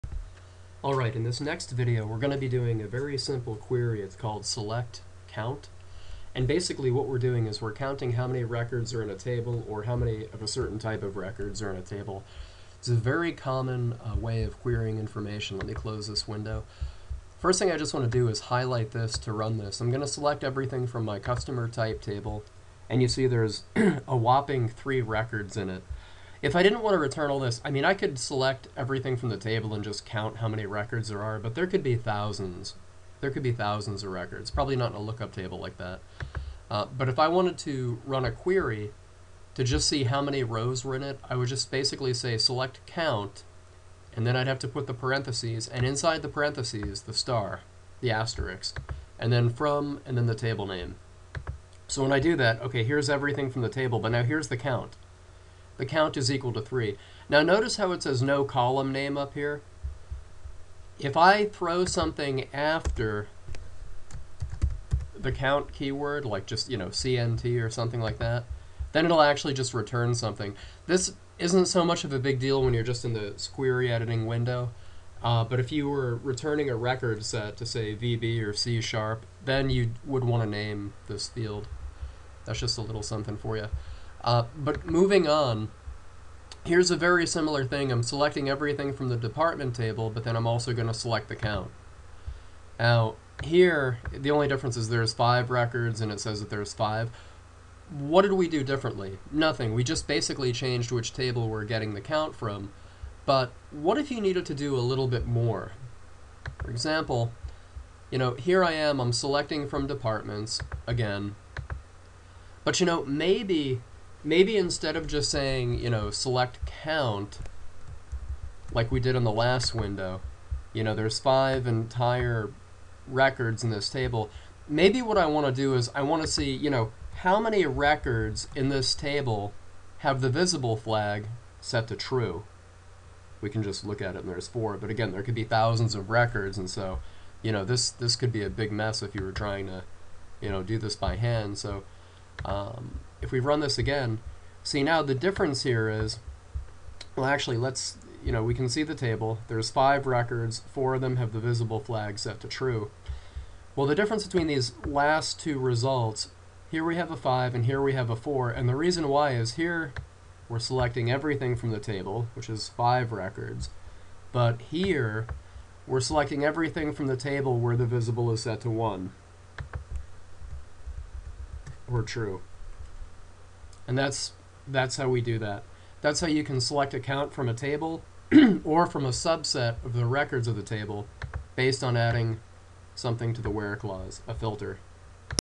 SQL Server - Query Table Record Data via TSQL - SELECT COUNT
How to select count(*) of table records. This tutorial covers how to select count(*) of entire table or how to select count(*) of a subset of data via using a where clause filter.